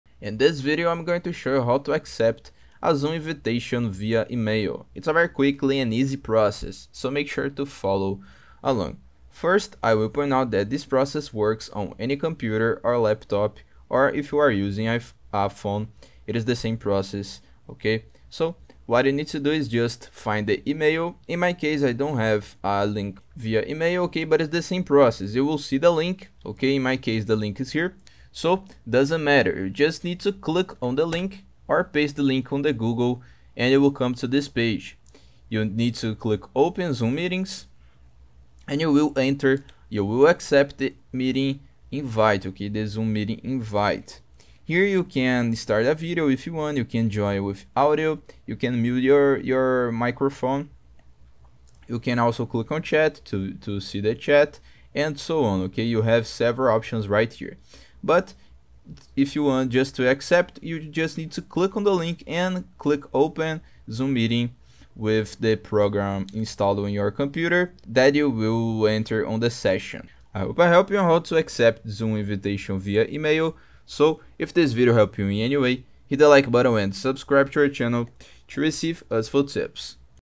How To Accept Zoom Invitation Via Email [EASY!]
In this video I will show you how to accept zoom invitation via email.

I hope I have helped you with the video!